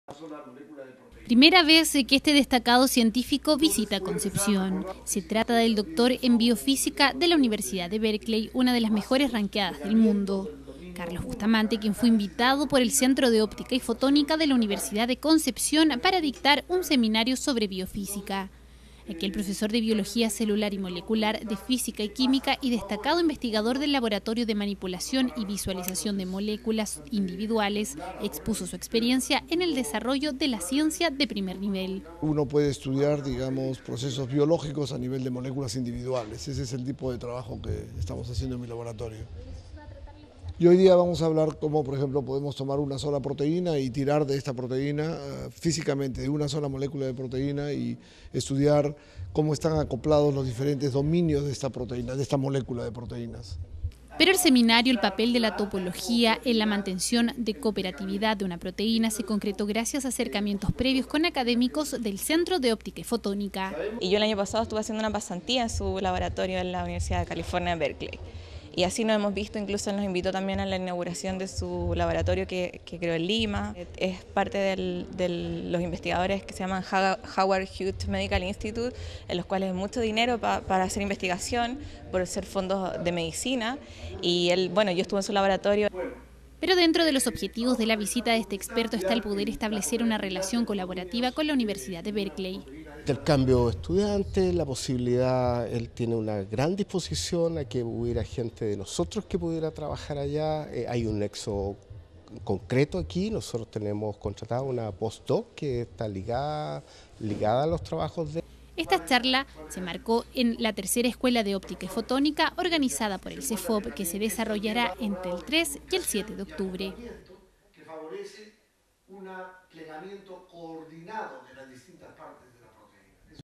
Centro de Óptica y Fotónica realiza seminario sobre biofísica
Carlos Bustamante, doctor en biofísica de la Universidad de Berkeley

Nota: TVU Noticias
Fecha: 27/09/2011